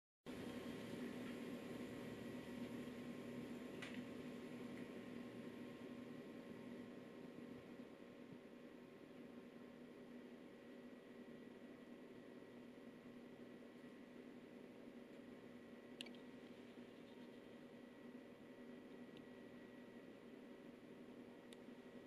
シダムシの幼生
棘皮動物のシダムシの幼生と思う。
漁港の水面で採集。
シダムシはヒトデ類の体内に寄生する。